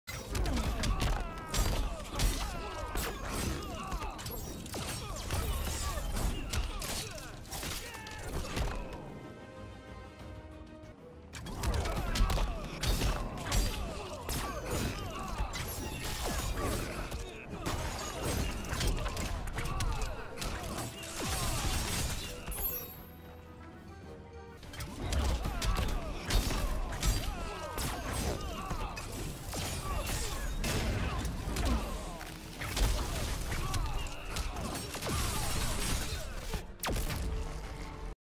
Brainiac Interactable Shenanigans (pre-patch)
It's hard to get interactable combos with power characters since there are so few that don't instantly knock the opponent down. But I've managed to find a few. Hope this helps. Enjoy!